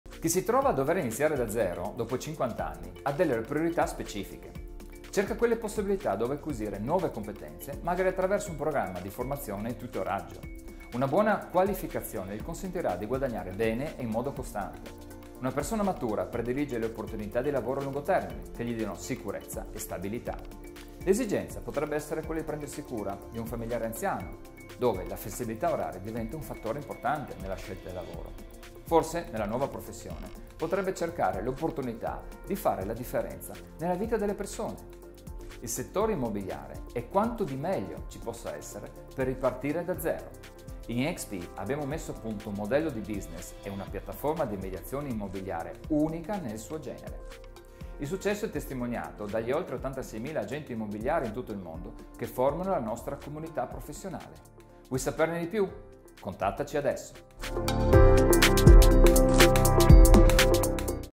Agente Immobiliare 50+
Ripartire dopo i 50 anni, una sfida personale e professionale che può essere vinta nel settore immobiliare.
Le persone in età matura hanno priorità ed esigenze diverse rispetto al resto della popolazione lavorativa.
In eXp lo sappiamo bene, e te lo raccontiamo in questo video.
Siamo curiosi di conoscerti, segui il link e fissa un appuntamento!
Sei un agente immobiliare e desideri ricevere più informazioni su eXp Italy?